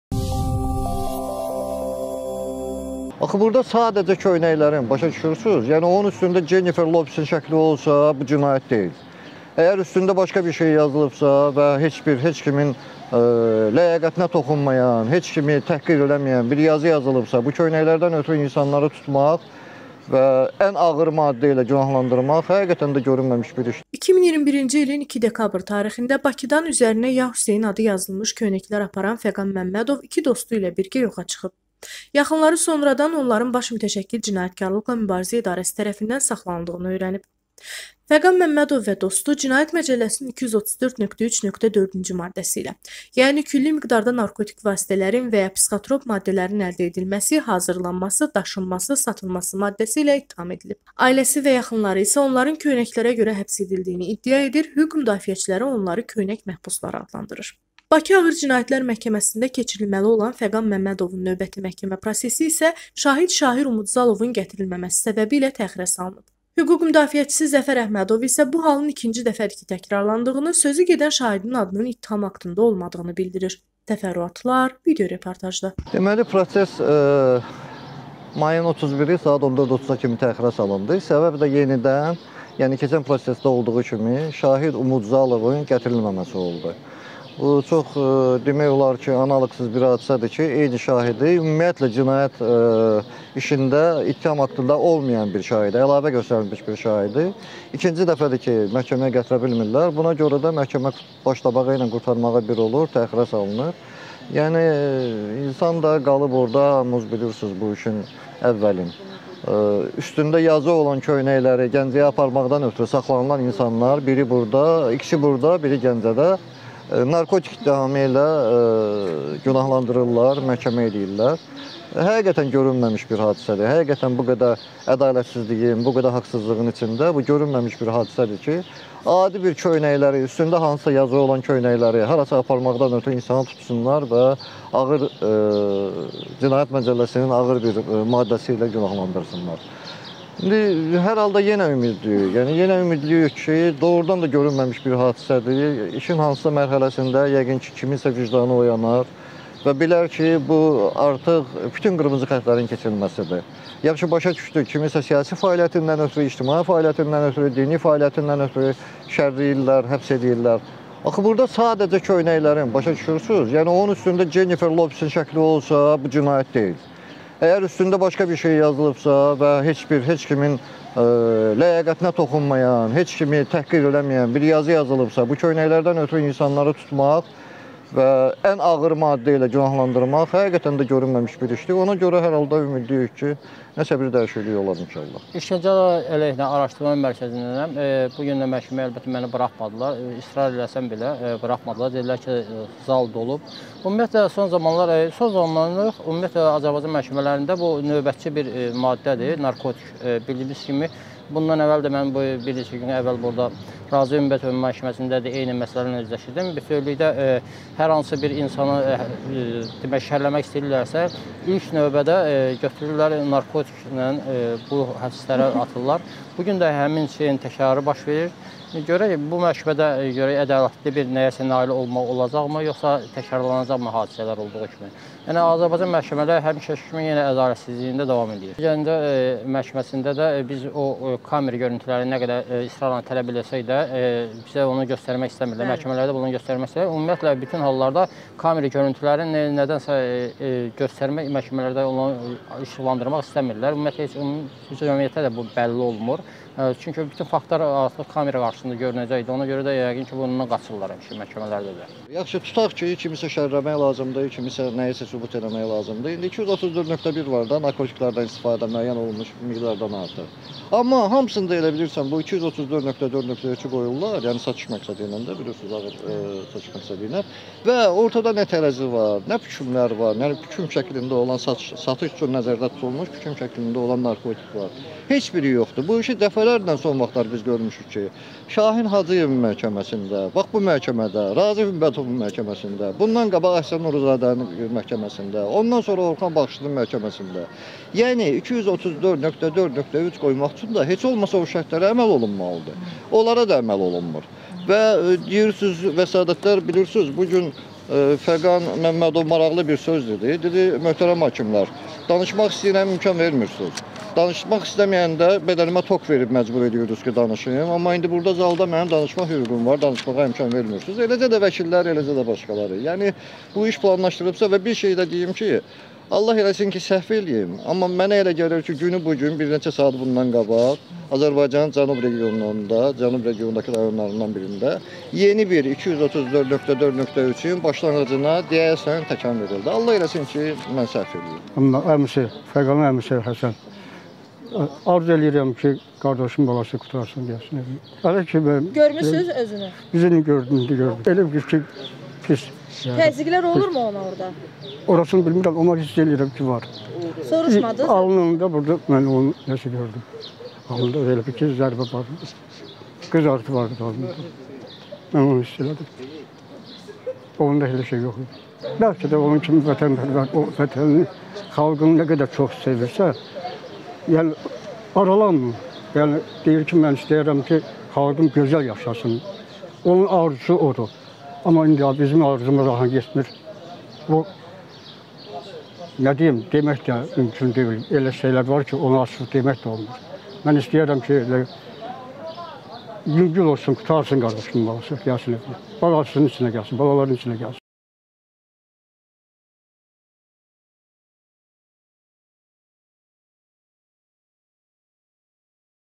"Adi köynəyə görə adamı TUTARLAR?, BU İŞ PLANLAŞDIRILIB"-"KÖYNƏK MƏHBUSLARI" NIN MƏHKƏMƏSİ
2021-ci ilin 2 dekabr tarixində Bakıdan üzərinə ya Hüseyn adı yazılmış köynəklər aparan Fəqan Məmmədov 2 dostu ilə birgə yoxa çıxıb. Yaxınları sonradan onların Baş Mütəşəkkil Cinayətkarlıqla Mübarizə İdarəsi tərəfindən saxlanıldığını öyrənib.

Fəqan Məmmədovu və dostu Azərbaycan Cinayət Məcəlləsinin 234.3.4-cü maddəsi ilə - külli miqdarda narkotik vasitələri və ya psixotrop maddələrin əldə edilməsi, hazırlanması, daşınması, satılması -maddəsi ilə ittiham ediblər. Ailəsi isə və yaxınları isə onların köynəklərə görə həbs edildiyini iddia edir, hüquq müdafiəçiləri onları köynək məhbusları adlandırır.
Bakı Ağır Cinayətlər məhkəməsində keçirilməli olan Fəqan Məmmədovun növbəti məhkəmə prosesi isə şahid Şahir Umudzalovun gətirilməməsi səbəbi ilə təxirə salınıb.
Hüquq müdafiəçisi Zəfər Əhmədov isə bu halın ikinci dəfədir ki, təkrarlandığını. Sözügedən şahidin adının ittiham aktında olmadığını bildirir. Təfərrüatlar video reportajda